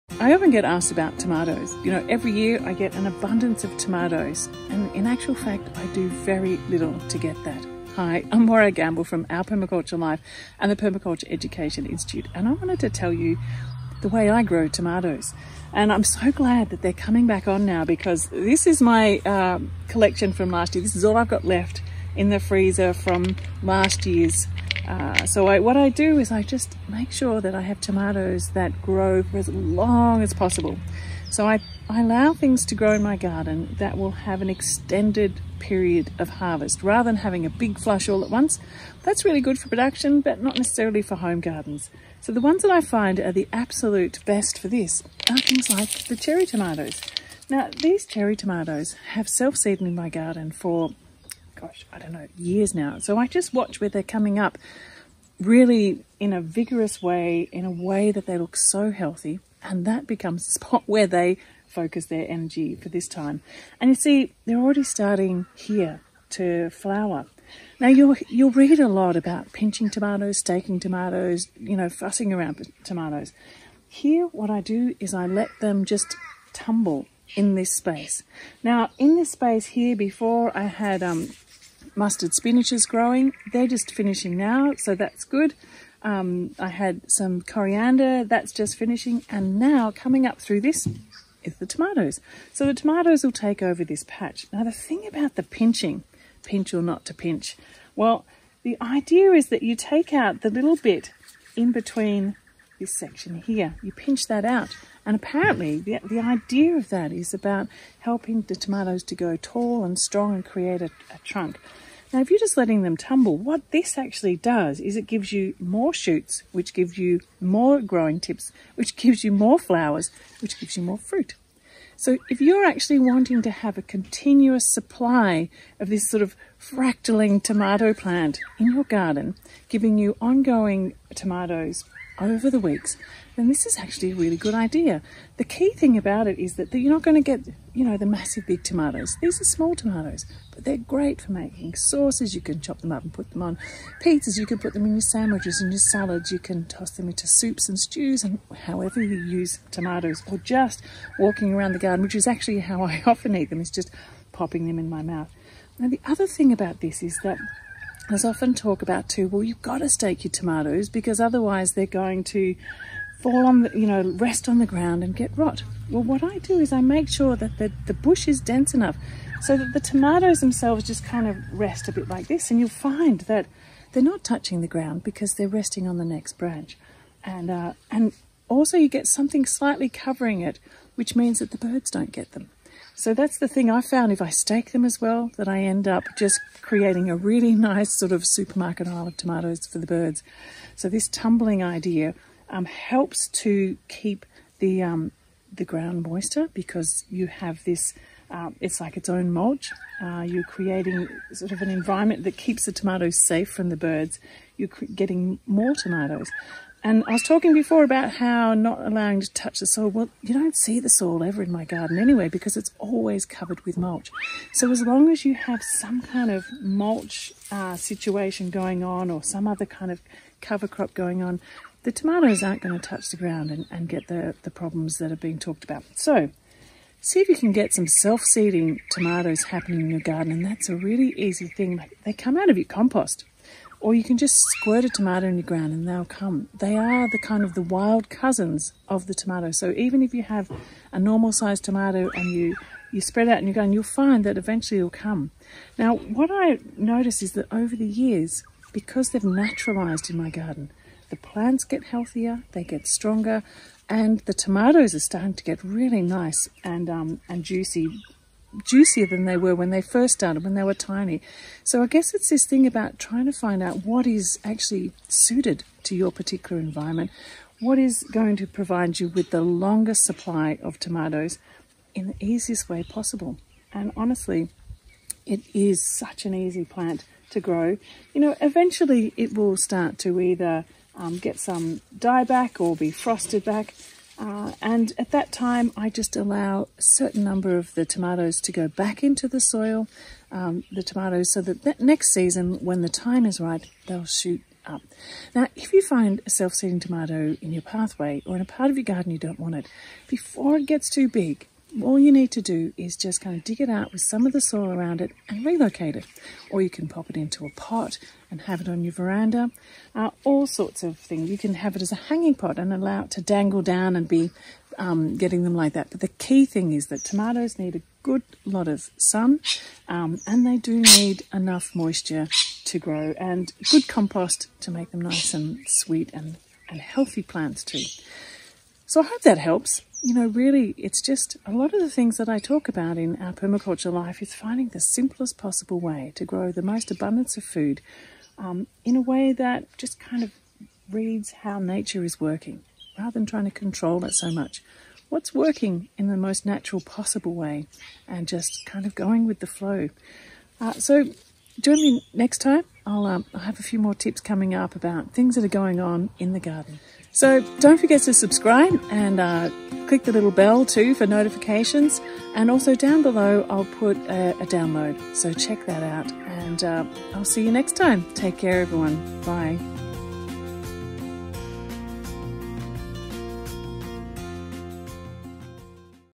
How to grow tomatoes simply with Morag Gamble
In this short video on 'How to grow tomatoes simply', Morag will show you how she grows tomatoes the simplest way she knows to get a long harvest of delicious hardy tomatoes with the least amount of challenges.